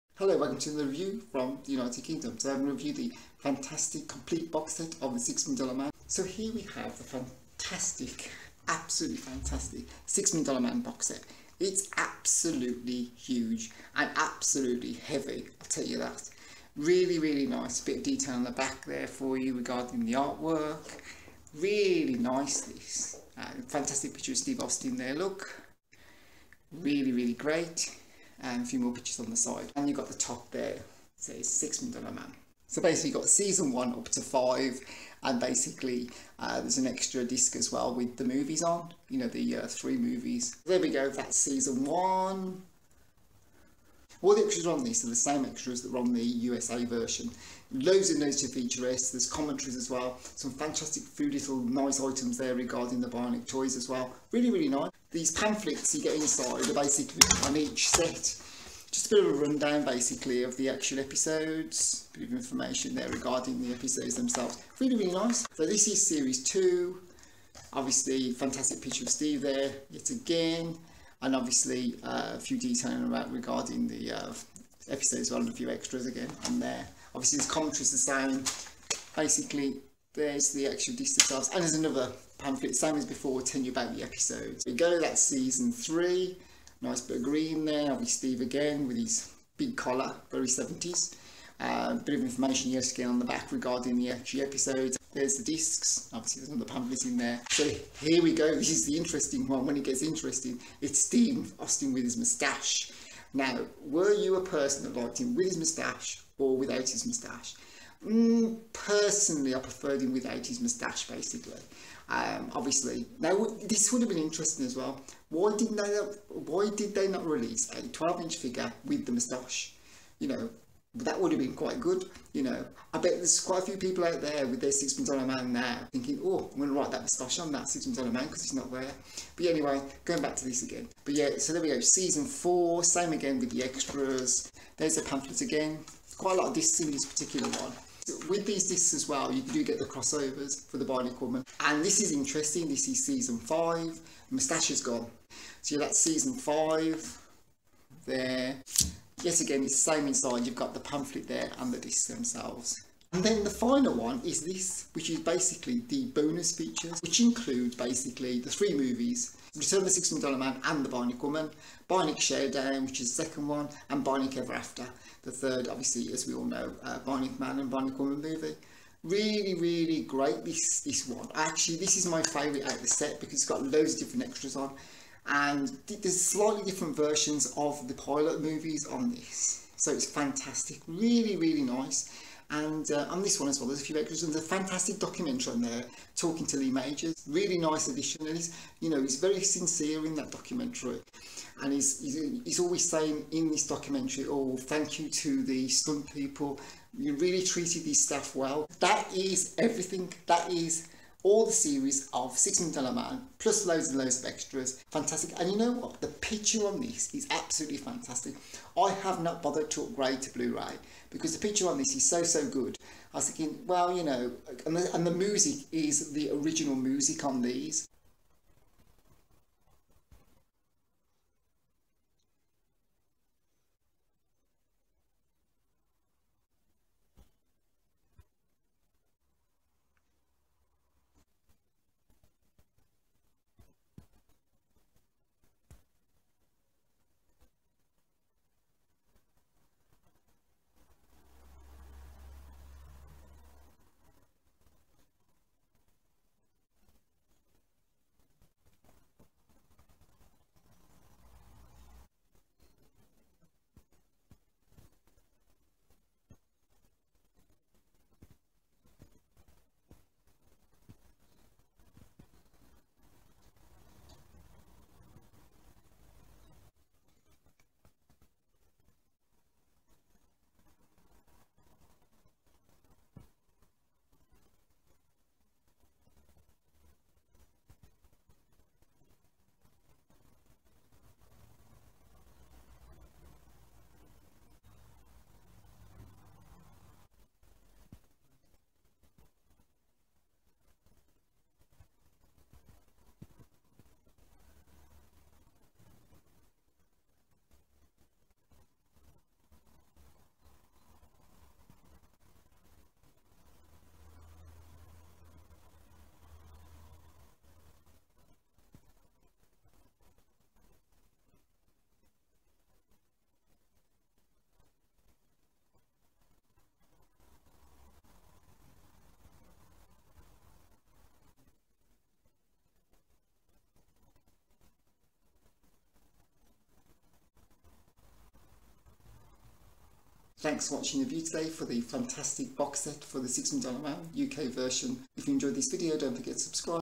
THE SIX MILLION DOLLAR MAN THE BIONIC MAN DVD BOXSET REVIEW LEE MAJORS DVD EXTRAS NOT ON BLU-RAY
DVD BOXSET REVIEW The Six Million Dollar Man ,The Bionic Man .One of the best sci-fi shows from the 1970s. I remember with great  fondness the television tv toy adverts , commercials for the Six Million Dollar Man . Kenner and Denys Fisher released The Six Million Dollar Man and The Bionic Woman toys in the 70s. FULL REVIEW OF THIS FANTASTIC DVD BOXSET . DVD EXTRAS NOT RELEASED ON BLU-RAY.